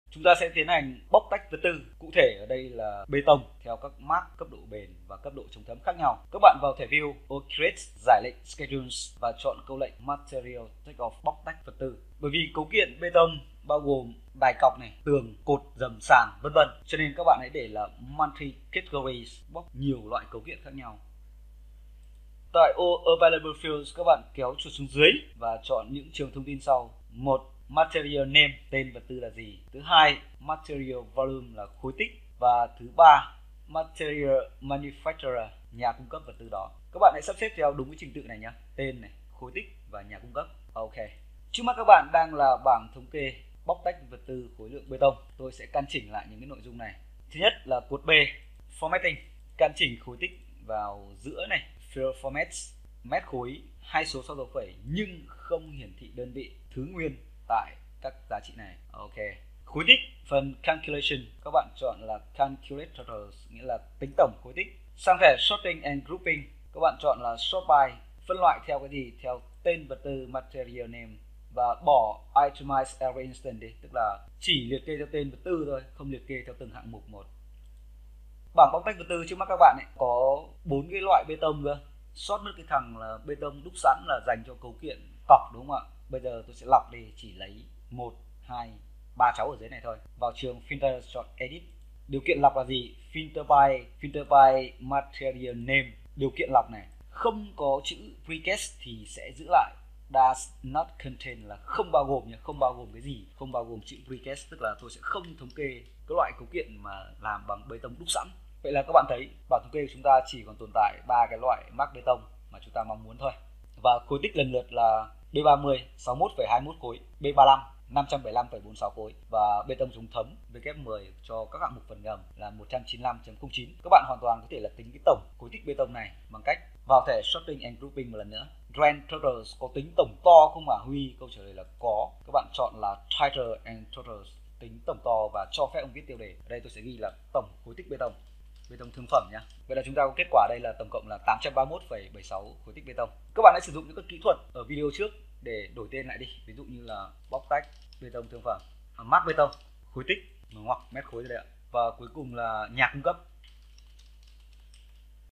Revit 2024| khóa học Revit Structure Bài 12 - Phần 12
Khóa học Revit Structure
Bài 12: Lập hồ sơ dự án (P1): Thống kê khối lượng – Bóc tách vật tư

Thống kê khối lượng BT & tổ chức dữ liệu bảng thống kê theo từng chủng loại cấu kiện (Móng – Cột – Vách – Dầm – Sàn)

Tạo mới chủng loại BT, bóc tách vật tư theo độ bền & mác chống thấm Material Takeoff

Thống kê thép cốt BT & kỹ thuật tổ hợp tham số cao cấp quản lý dữ liệu bảng thống kê cốt thép

Để xem thông tin chi tiết về khóa học, hãy truy cập vào đường